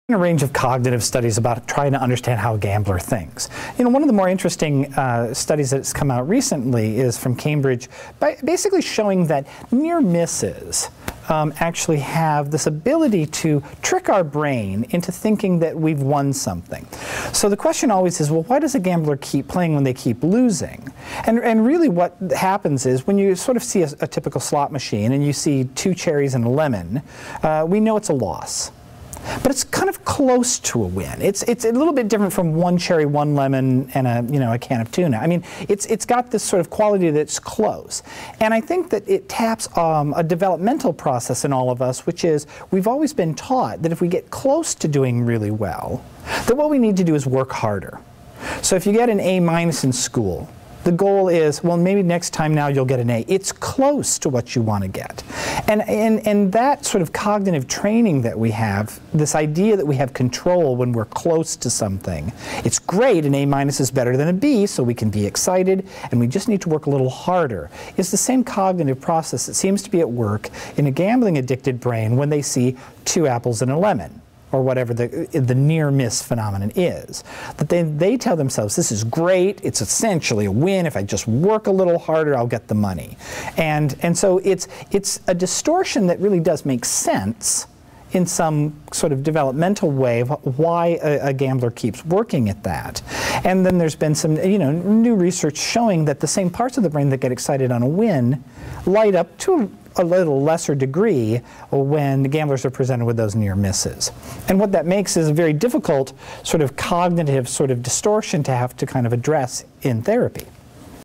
Near Misses in Pathological Gambling May Trick the Brain
Dr. Jon Grant discusses how research suggests that "near misses" in gambling may have the ability to trick our brain into thinking that the loss can be turned into a win if the gambler just "tries harder." Research using neuroimaging suggests cortical activation in brain regions associated with wins are also activated, to a slightly lesser degree when pathological gamblers encounter "near misses" and thus, it may mimic the illusion that one is successful in gambling behavior where losses are obtained. These patterns may influence irrational thinking among pathological gamblers.

Research article cited by Dr. Grant: Clark, L., Lawrence, A. J., Astley-Jones, F. & Gray, N. 2009 Gambling near-misses enhance motivation to gamble and recruit win-related brain circuitry. Neuron 61, 481--490.